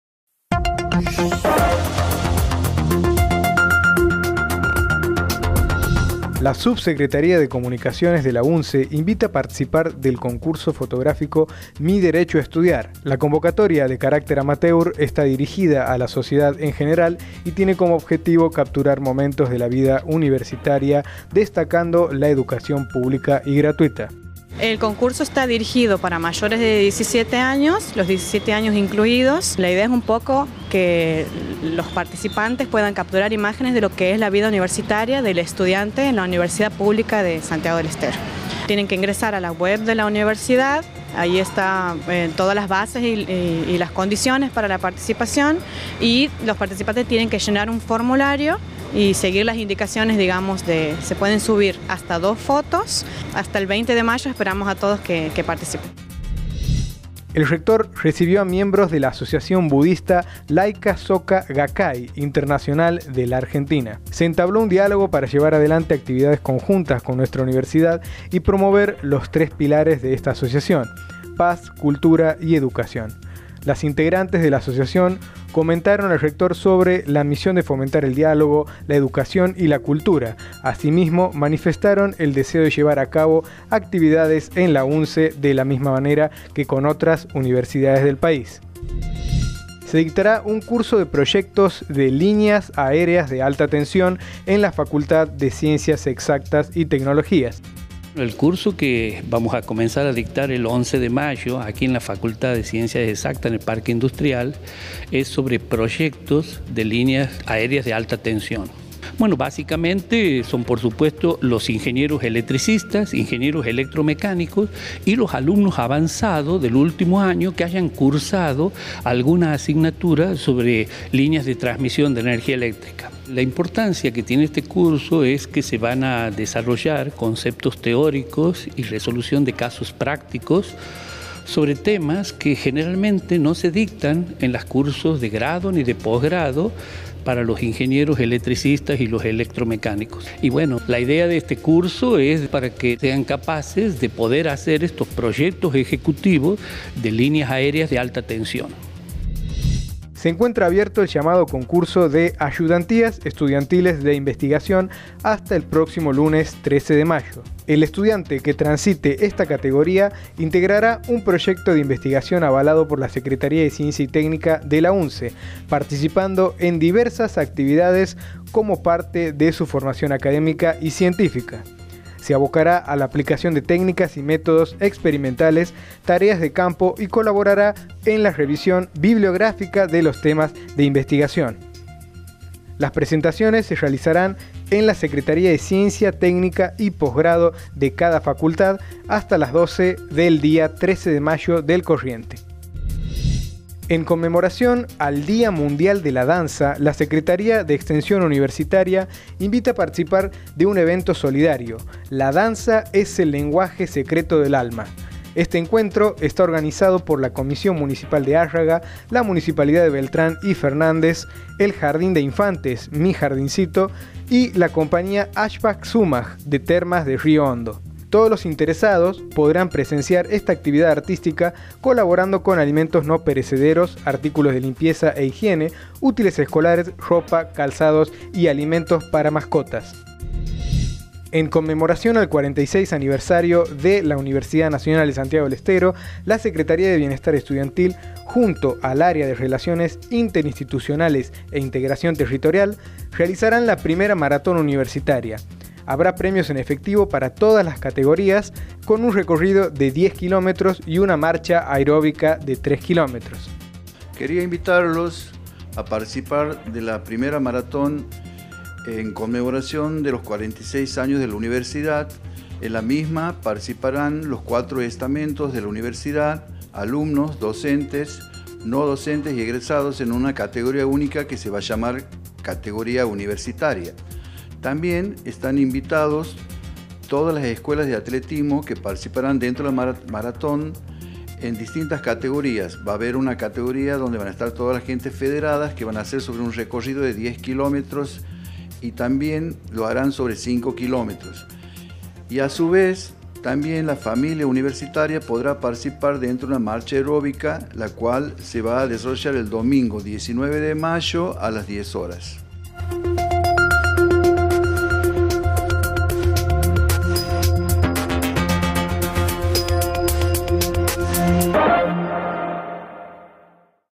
INFO UNSE 03-05-2019
Compartimos el INFO UNSE de este viernes 3 de mayo:

* Concurso fotográfico: “Mi derecho a estudiar”.
* Asociación Budista Laica Soka Gakkai Internacional de la Argentina.
* Curso sobre Líneas Aéreas de Alta Tensión.
* Convocatoria para Ayudantías Estudiantiles de Investigación.
* Evento solidario “La danza es el lenguaje secreto del alma”, por el Día Mundial de la Danza.
* Primera Maratón Universitaria por el 46° Aniversario UNSE.